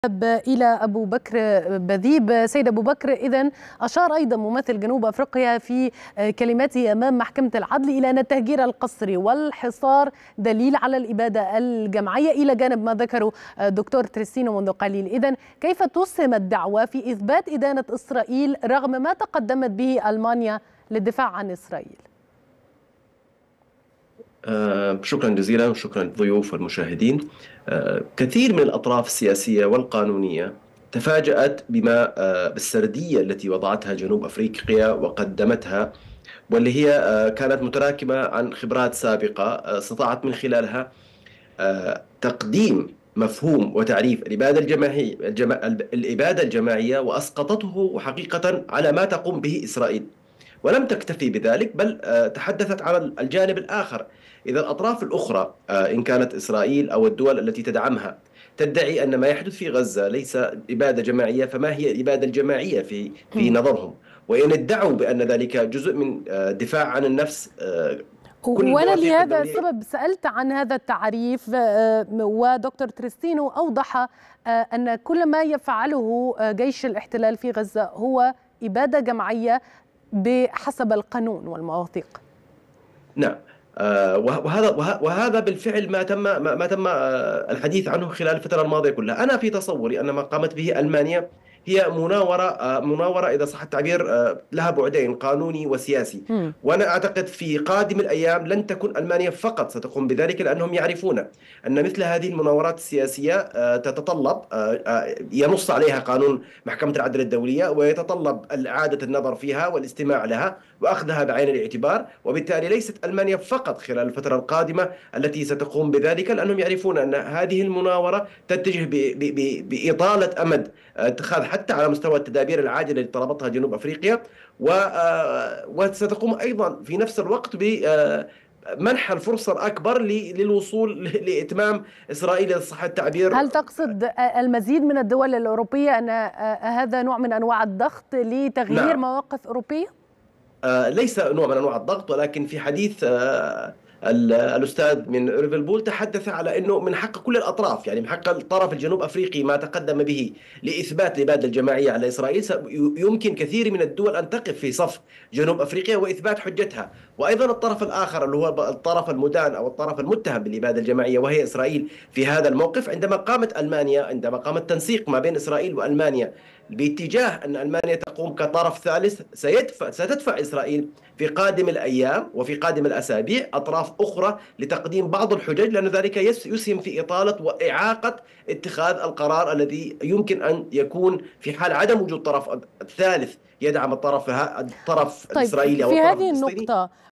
مناورة ألمانيا لمساندة إسرائيل أمام محكمة العدل الدولية.. باحث في الشؤون الاستراتيجية يكشف التفاصيل
أكد أبوبكر باذيب، الباحث في الشؤون الاستراتيجية، أن ما قامت به ألمانيا من مساندة إسرائيل في اتهامها بارتكاب جرائم الإبادة الجماعية في قطاع غزة، هو مناورة سياسية وقانونية لأن هذا سيزيد من مدة اتخاذ القرارات، وسيمنح الفرصة لدول أخرى للدخول كطرف ثالث في تلك القضية.

17/1/2024